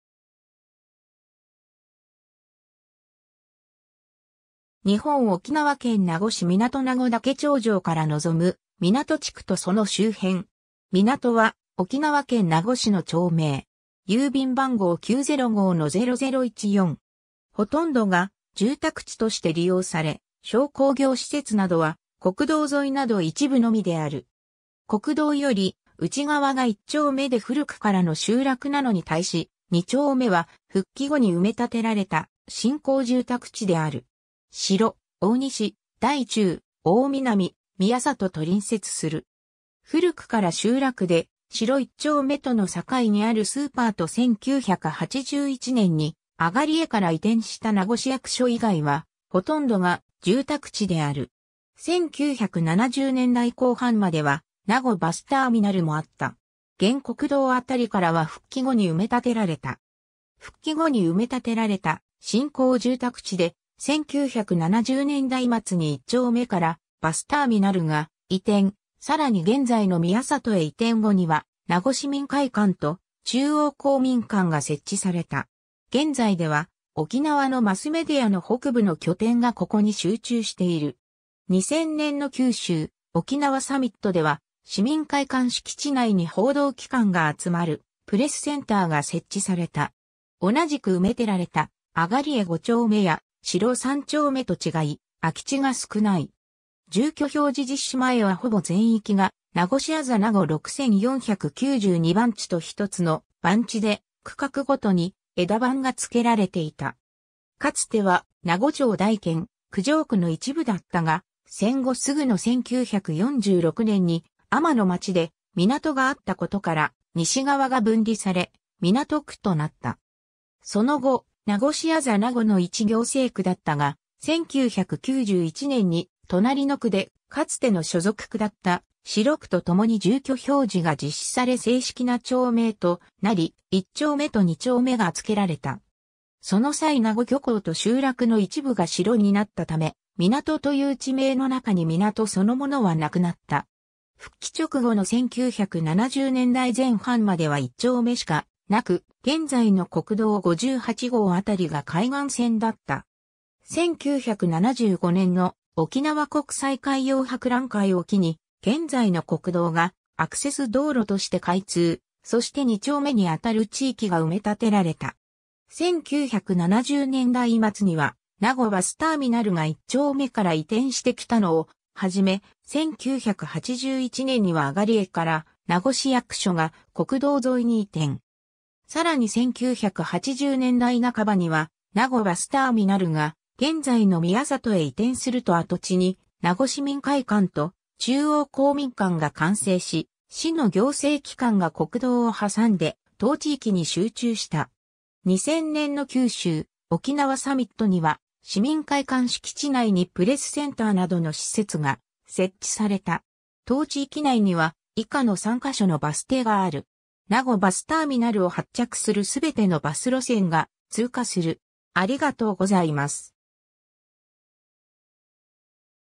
港 (名護市)
名護市の町・字
日本 」 沖縄県 」 名護市 」 港 (名護市) 名護岳頂上から望む港地区とその周辺。
 港（みなと）は、沖縄県名護市の町名。
 ほとんどが住宅地として利用され、商工業施設などは国道沿いなど一部のみである。
国道より内側が1丁目で古くからの集落なのに対し、2丁目は復帰後に埋め立てられた新興住宅地である。
城・大西・大中・大南・宮里と隣接する。
 古くから集落で、城1丁目との境にあるスーパーと1981年に東江から移転した名護市役所以外はほとんどが住宅地である。
1970年代後半までは名護バスターミナルもあった。
現国道あたりからは復帰後に埋め立てられた。
 復帰後に埋め立てられた新興住宅地で、1970年代末に1丁目からバスターミナルが移転、さらに現在の宮里へ移転後には名護市民会館と中央公民館が設置された。
現在では沖縄のマスメディアの北部の拠点がここに集中している。
2000年の九州・沖縄サミットでは市民会館敷地内に報道機関が集まるプレスセンターが設置された。
同じく埋めてられた東江5丁目や城3丁目（名護漁港内）と違い、空き地が少ない。
住居表示実施前はほぼ全域が「名護市字名護6492番地」と１つの番地で区画ごとに枝番がつけられていた。
 かつては名護町大兼久城区の一部だったが、戦後すぐの1946年に海人（ウミンチュ、沖縄方言で漁師の意）の町で港があったことから西側が分離され、港区となった。
 その後、名護市字名護の一行政区だったが、1991年に隣の区でかつての所属区だった城区とともに住居表示が実施され正式な町名となり、1丁目と2丁目がつけられた。
その際名護漁港と集落の一部が城になったため、「港」という地名の中に港そのものはなくなった。
 復帰直後の1970年代前半までは1丁目しかなく、現在の国道58号辺りが海岸線だった。
1975年の沖縄国際海洋博覧会を機に現在の国道がアクセス道路（当時は県道本部循環線のバイパス）として開通、そして2丁目にあたる地域が埋め立てられた。
1970年代末には名護バスターミナルが1丁目から移転してきたのをはじめ、1981年には東江から名護市役所が国道沿いに移転。
さらに1980年代半ばには名護バスターミナルが現在の宮里へ移転すると跡地に名護市民会館と中央公民館が完成し、市の行政機関が国道を挟んで当地域に集中した。
2000年の九州・沖縄サミットには市民会館敷地内にプレスセンターなどの施設が設置された。
 当地域内には以下の3ヶ所のバス停がある（カッコ内の数字は停車するバスの系統番号）。
 名護バスターミナルを発着するすべてのバス路線が通過する。